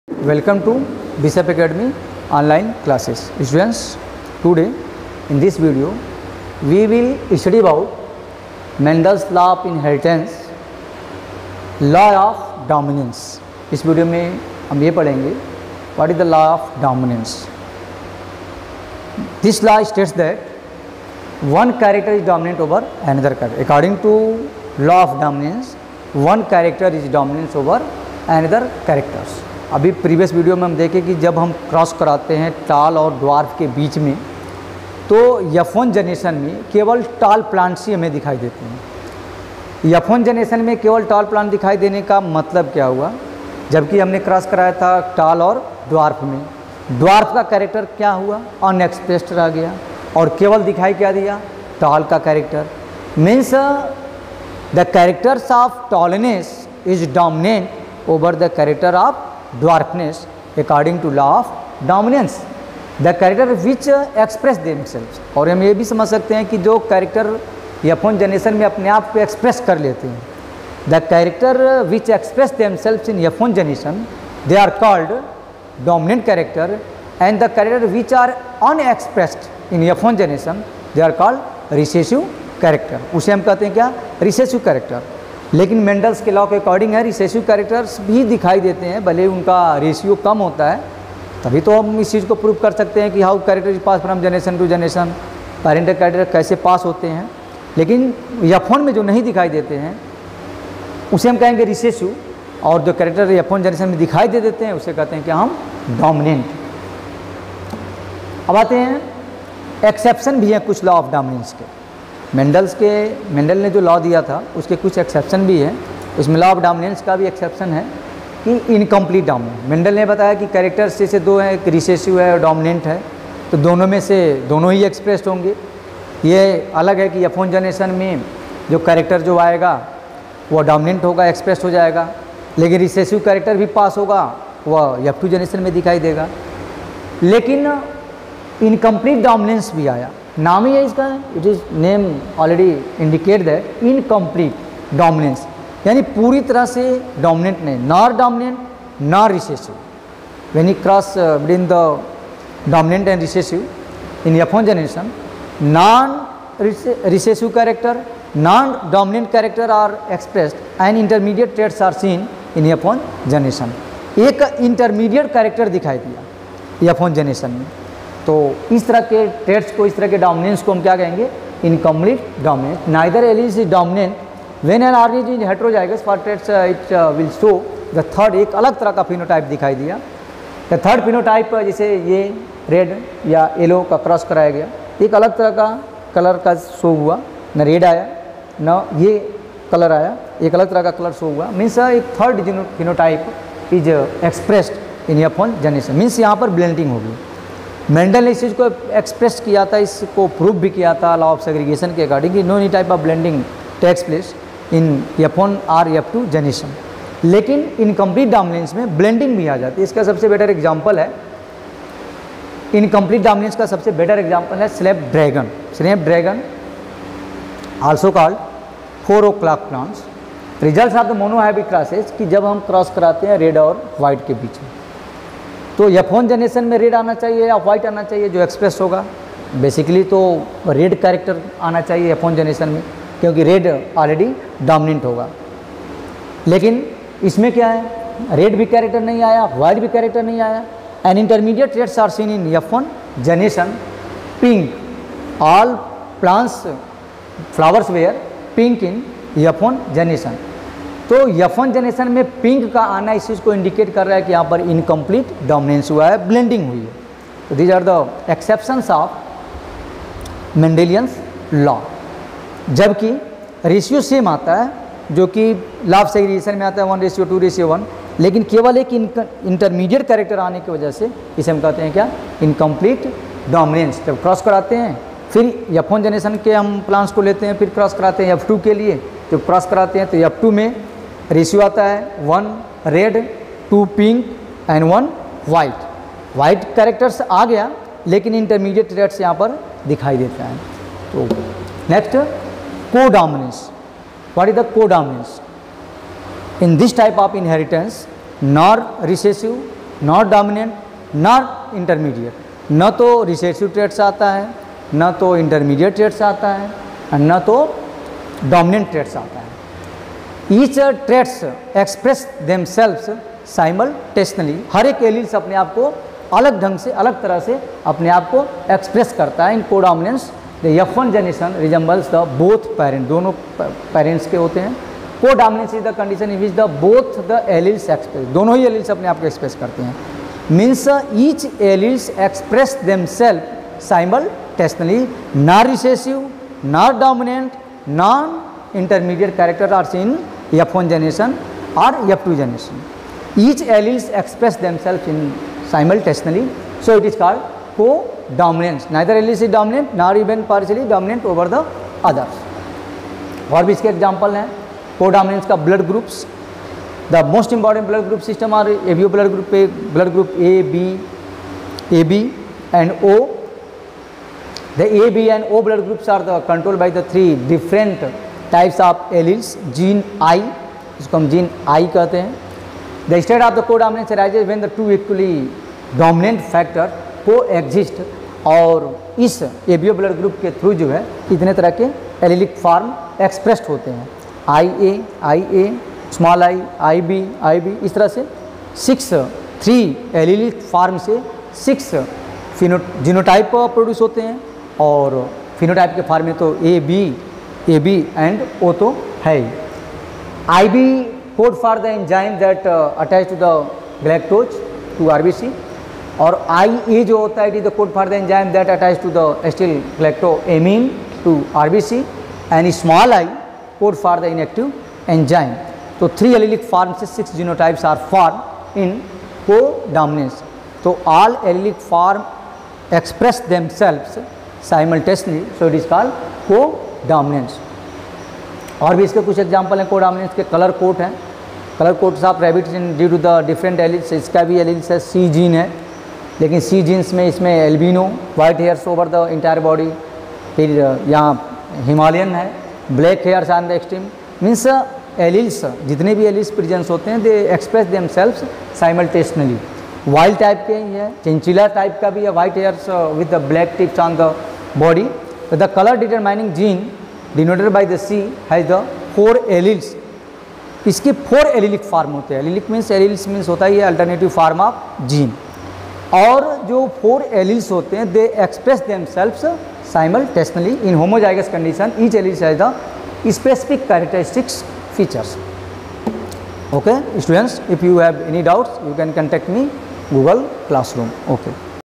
BIOLOGY | CLASS 12 CBSE | MENDAL'S LAW OF INHERITANCE (PART 3)
THIS VIDEO IS ADDRESS BY RAJEEV SRIVASTAVA SIR (FACULTY OF BIOLOGY)